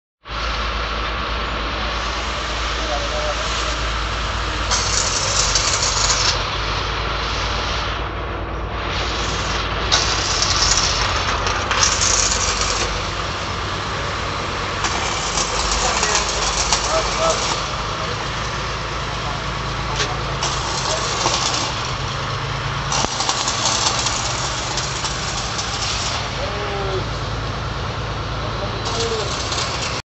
Chhuchhakwas ki trolley, Kamani wali trolley, Jack wali trolley, 9813646225, Ved Engineering
This Dumper is built for Bhatta owner of Bhatiyana. Can carry 5000 bricks at a time.